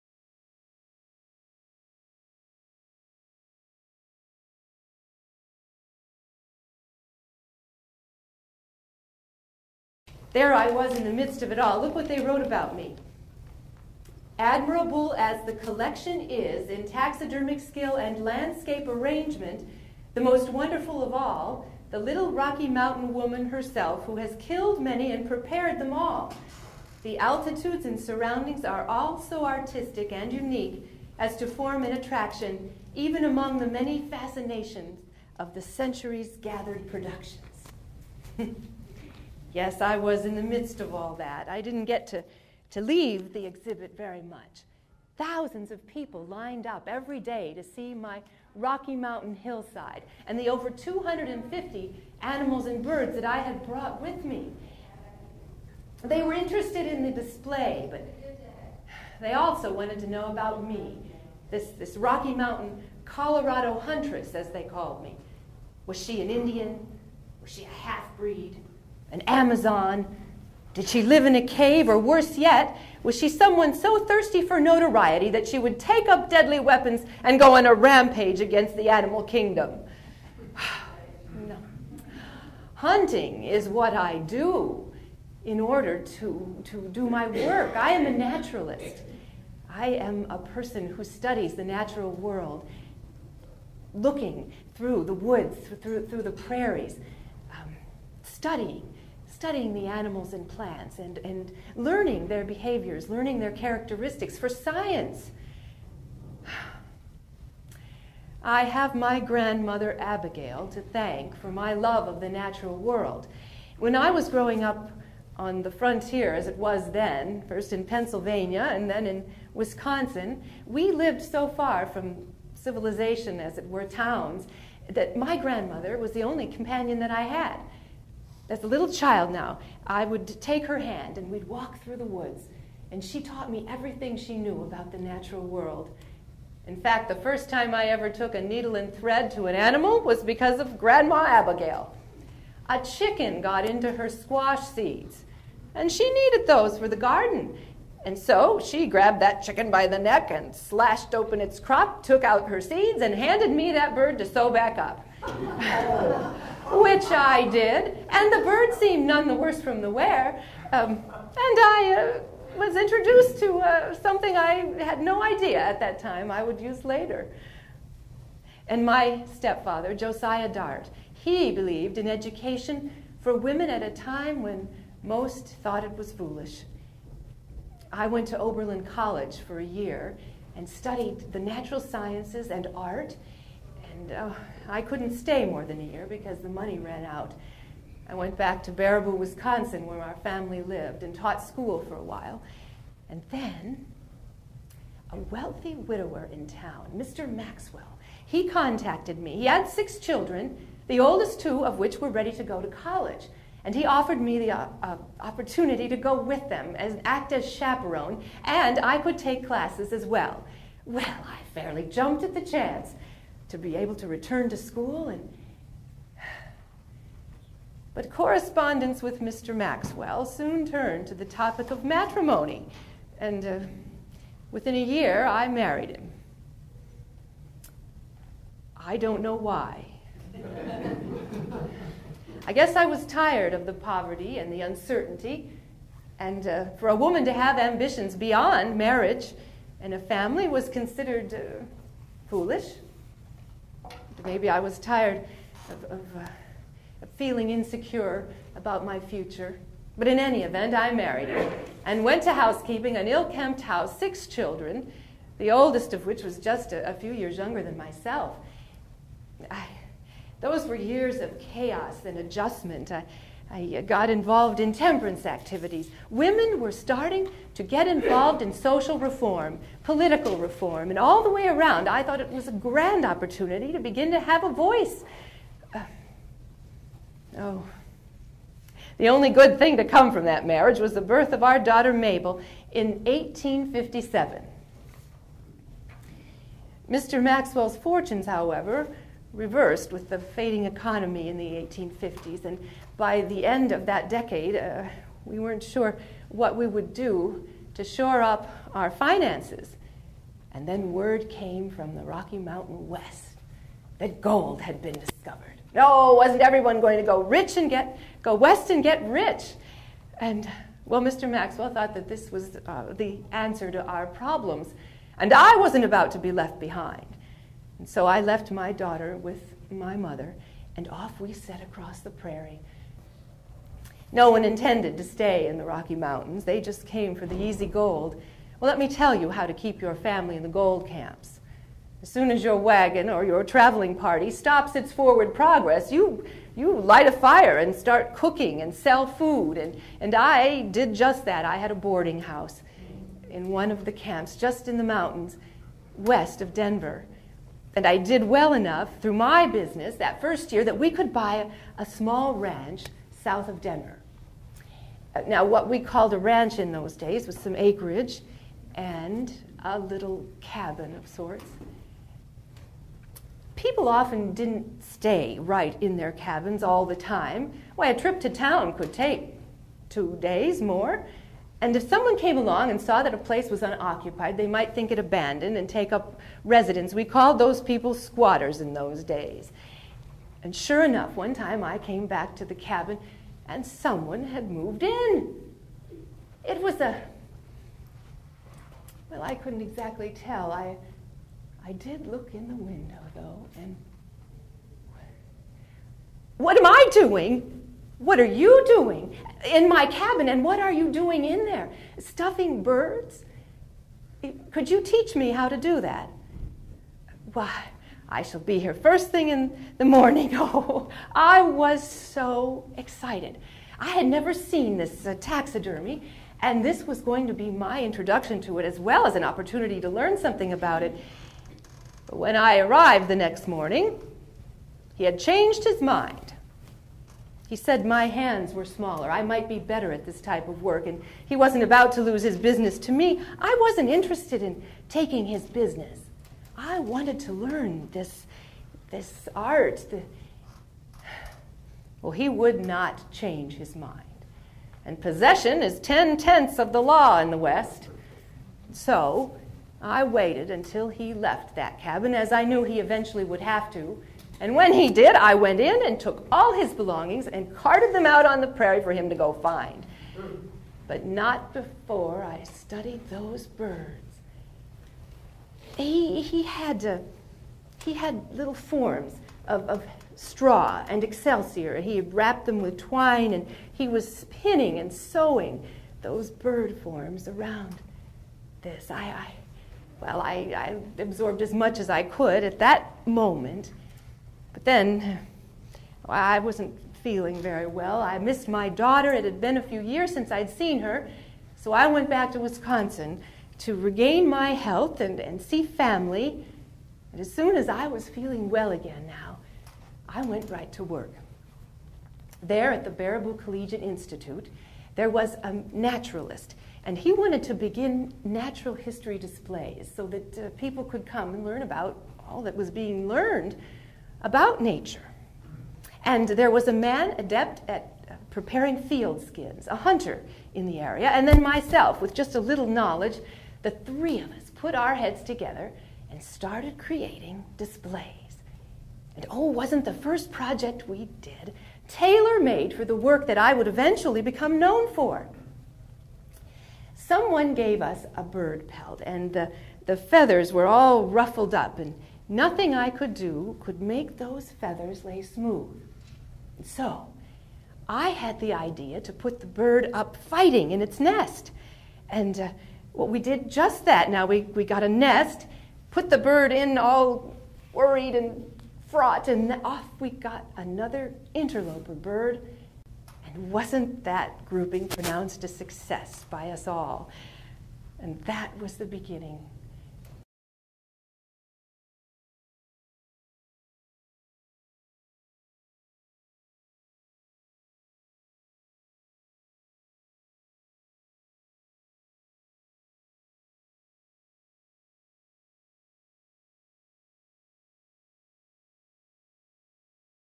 Martha Maxwell visits the National Cowboy & Western Heritage Museum
Martha Maxwell, portrayed by Mary Jane Bradbury, visited the National Cowboy & Western Heritage Museum on May 19, 2009 for a "Tuesdays at Sundown" program.

Martha Ann (Dartt) Maxwell (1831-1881) was the first woman field naturalist who obtained and prepared her own specimens in the same manner as her male contemporaries. As a taxidermist, Martha innovated the natural habitat diorama by placing stuffed animals in natural poses in detailed realistic habitats and directly influenced the designs of later museum dioramas. Her presentation of Colorado animals at the Philadelphia Centennial Exposition in 1876 did much to dispel the stereotypical notions about womens work.

Between January 16 and July 12, 2009, an exhibit titled "'Did She Kill 'em All?' Martha Maxwell, Colorado Huntress" is in the National Cowboy Museums Osborn Photography Studio Gallery in Prosperity Junction will explore Maxwells life and career. Three-dimensional images called anaglyphs, derived from contemporary stereographic images of Maxwell preserved in the Dickinson Research Center, will highlight and add another, so-to-speak, dimension to the exhibit. Additionally, a virtual three-dimensional exhibit will be available online in the Centers pages of the Museums web site.

Video: Adobe flash file